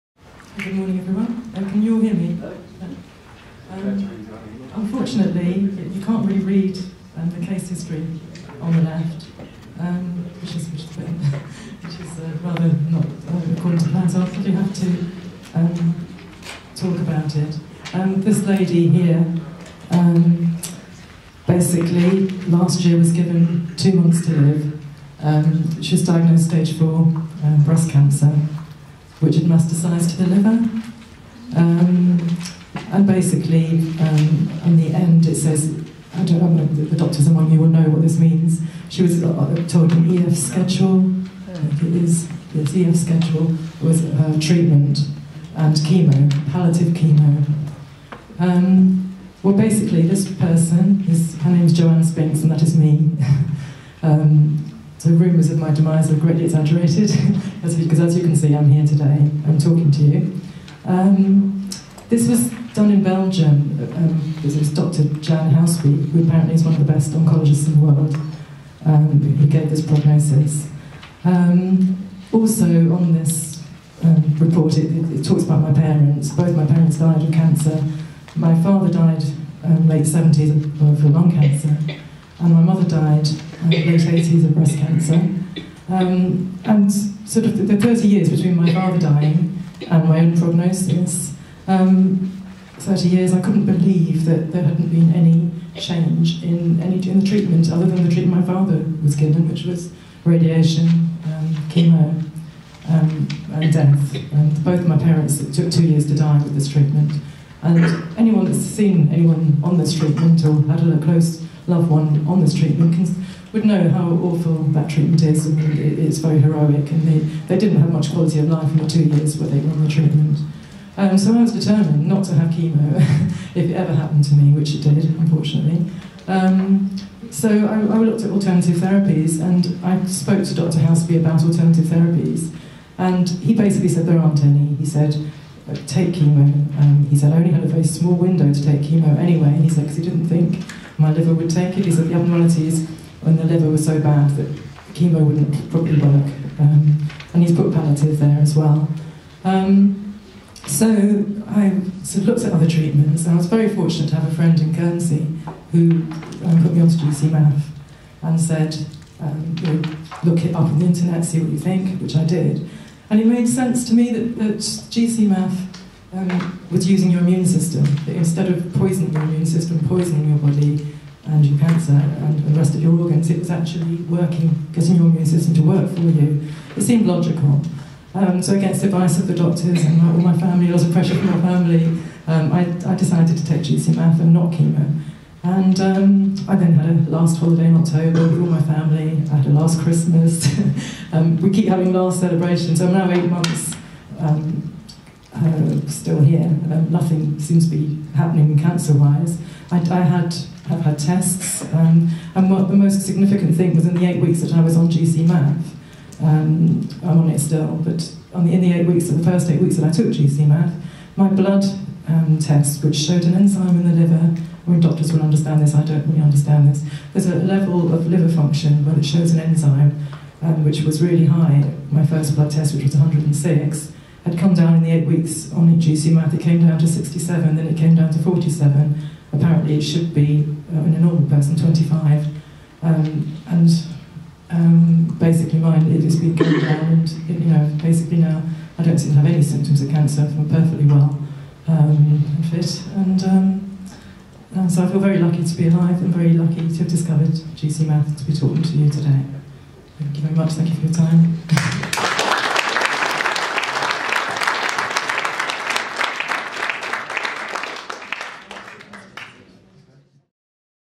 Intervento del dott.ssa Spinks al convegno mondiale sul Gcmaf a Francoforte
Intervento del dott.ssa Spinks al convegno mondiale sul Gcmaf a Francoforte tenutosi ad aprile 2013  sponsorizzato dalla azienda First Immune  by Immunobiotech.